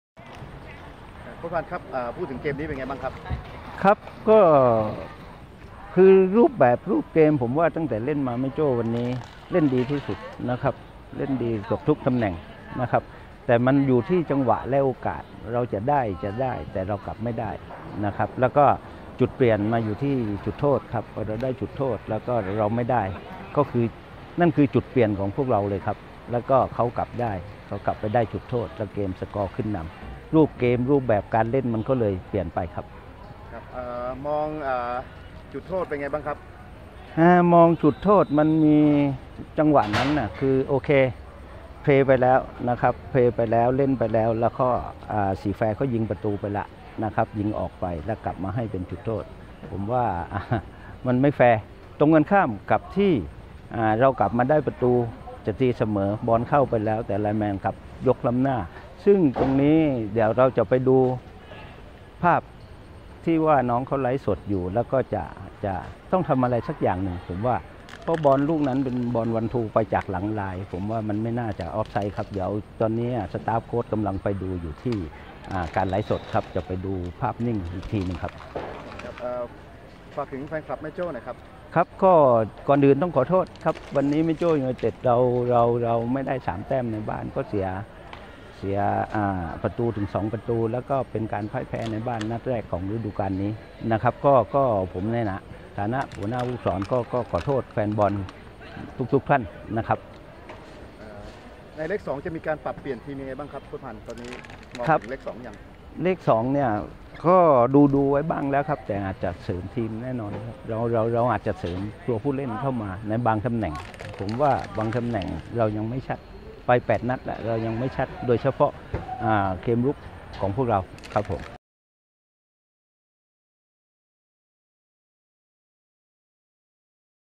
INT ชลาพันธ์ โค้ชแม่โจ้ หลังพ่ายสี่แคว 0-2
เครดิต นพดล นามวงค์พรหม